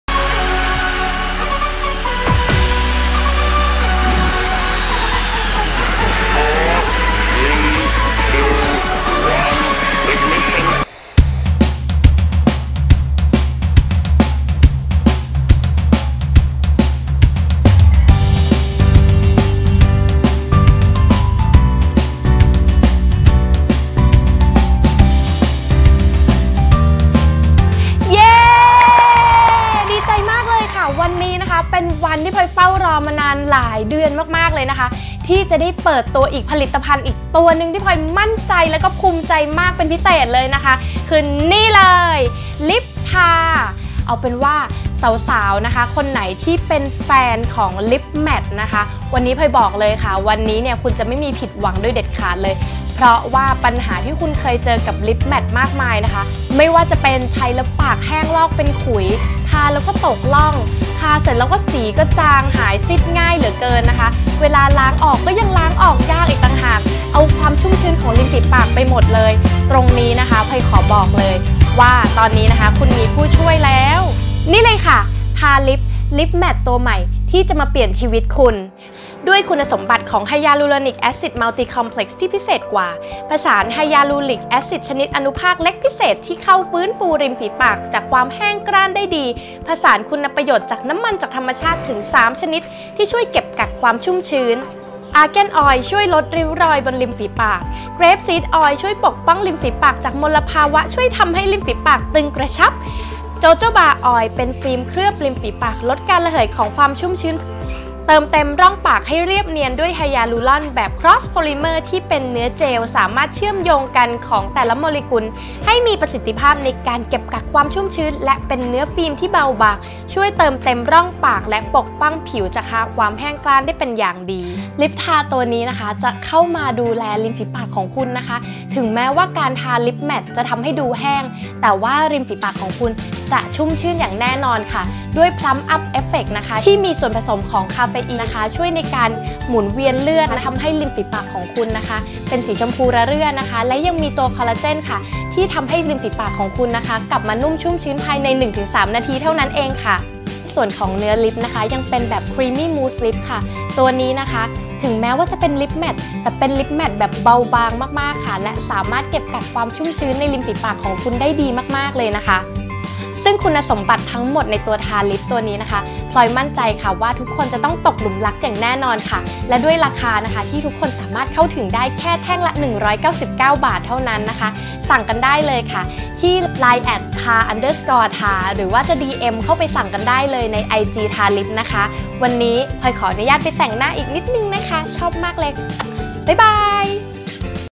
Thar Lip: Creamy Mousse Lip ลิปแมทยุคใหม่
Thar Lip ลิปแมทเนื้อมู้สบางเบา ติดทนนาน ไม่เป็นคราบเป็นขุย บำรุงเรียวปากให้นุ่มชุ่มชื้นด้วยน้ำมัน Argan Oil, Grape Seed Oil, Jojoba Oil และวิตามิน A&E